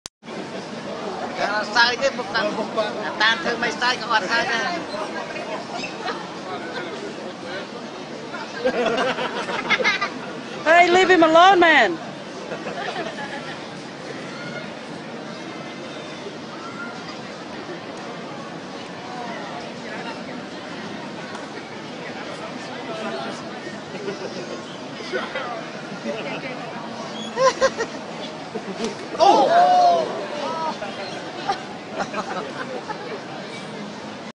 man punched by a fake statue for giving him a wet willy
he got it coming